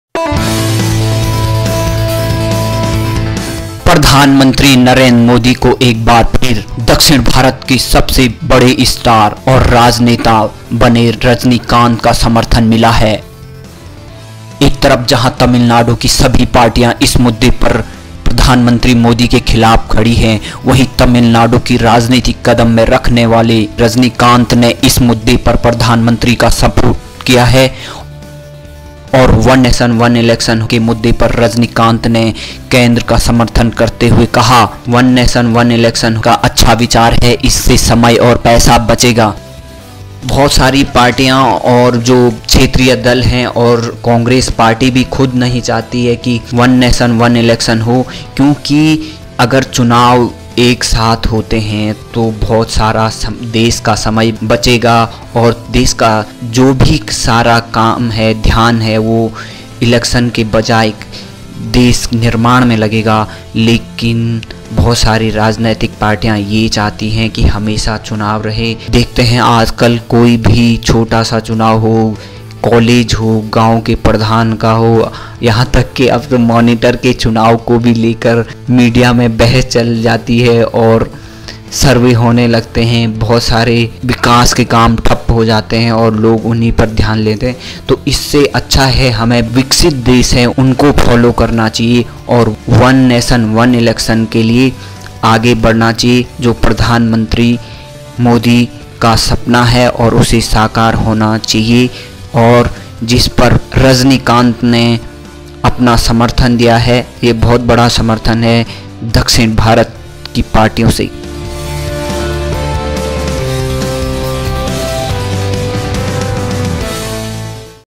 पीएम Narendra Modi को मिला सुपरस्टार Rajinikanth का समर्थन ! कही बड़ी बात
I support One Nation One Election. This will save money and time: Rajinikanth in Chennai Tamilnadu